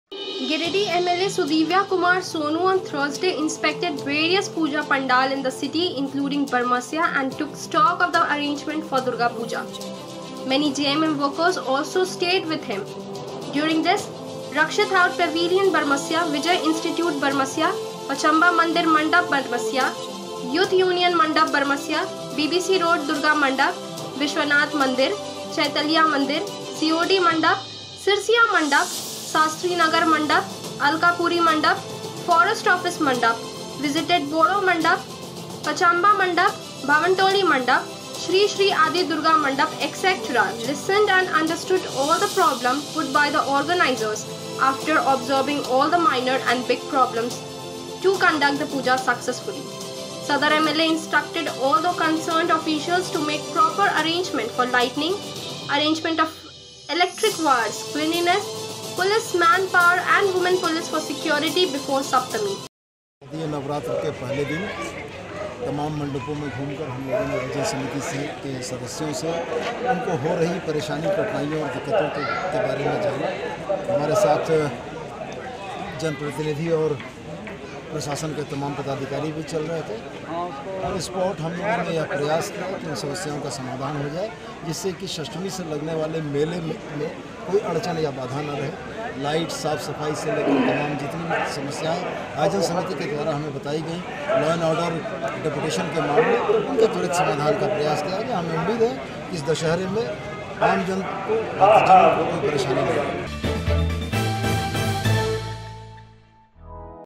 Giridih Sadar MLA Sudivya Kumar Sonu inspected various puja pavilions of the city
Sadar MLA Sudivya Kumar Sonu inspected various puja pavilions of the city on Thursday and gave necessary directions to the officials to maintain the system.